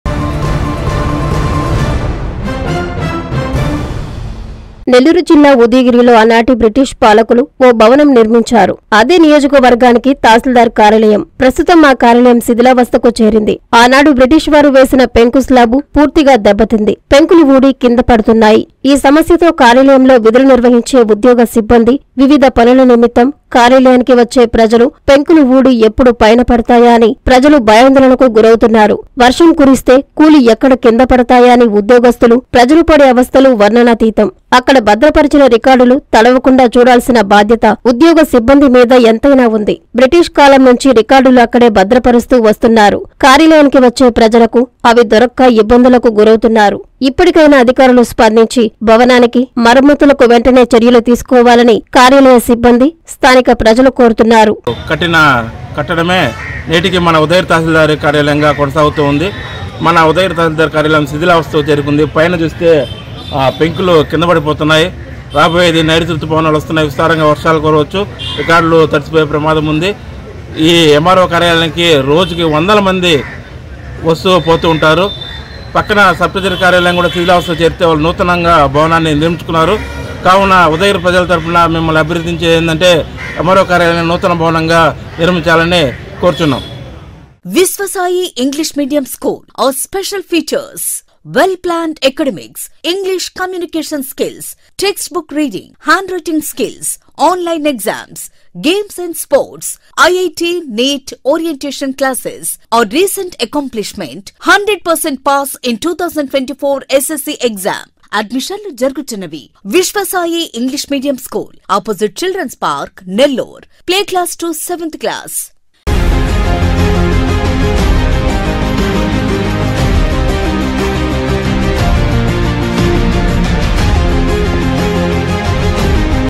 శిథిలావ‌స్థ‌లో ఉద‌య‌గిరి త‌హ‌సీల్దార్ కార్యాల‌యం
- వ‌ర్షం వ‌స్తే రికార్డులు భ‌ద్ర‌మేనా...?
- ఎప్పుడు కూలిపోతుందోన‌న్న భ‌యాందోళ‌న‌లో సిబ్బంది, ప్ర‌జ‌లు
- ఆనాటి బ్రిటిష్ పాలకుల భవనమే నేడు తాసిల్దార్ కార్యాలయం...!